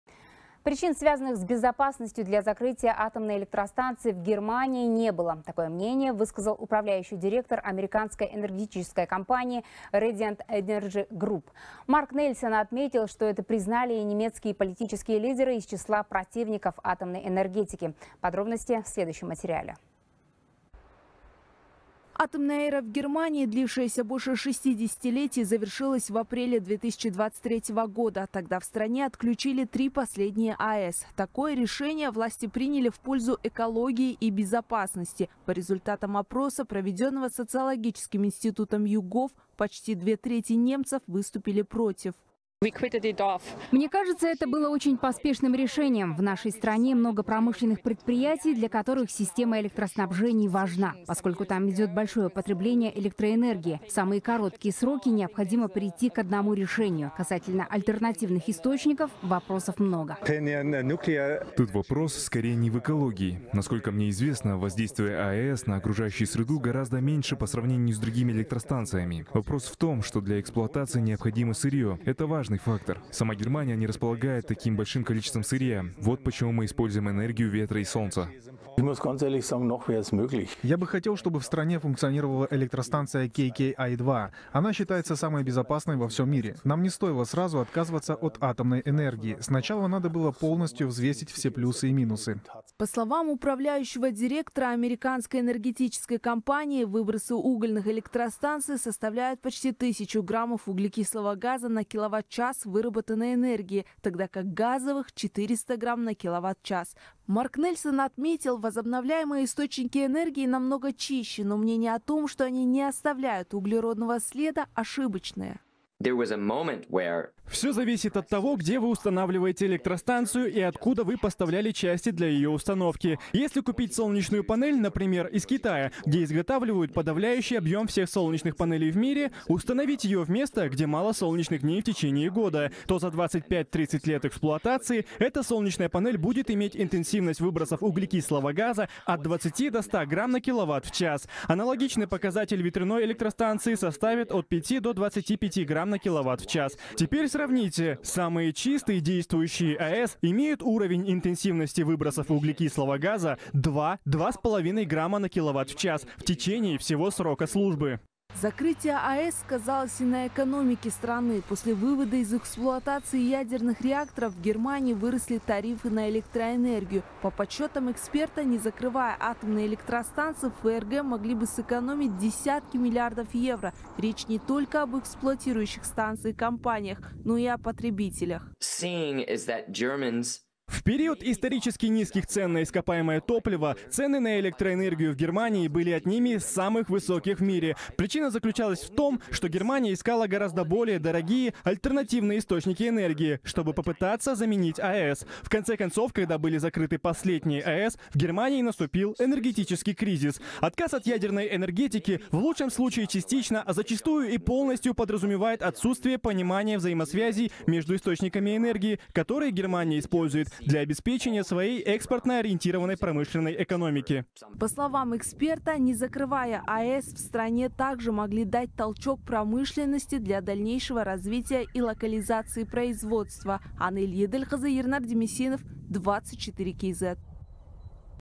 Последствия отказа от атомной энергии в Германии
Причин, связанных с безопасностью для закрытия Атомной электростанции в Германии не было. Такое мнение высказал управляющий директор американской энергетической компании Radiant Energy Group. Марк Нельсон отметил, что это признали и немецкие политические лидеры из числа противников атомной энергетики, передаёт корреспондент «24KZ».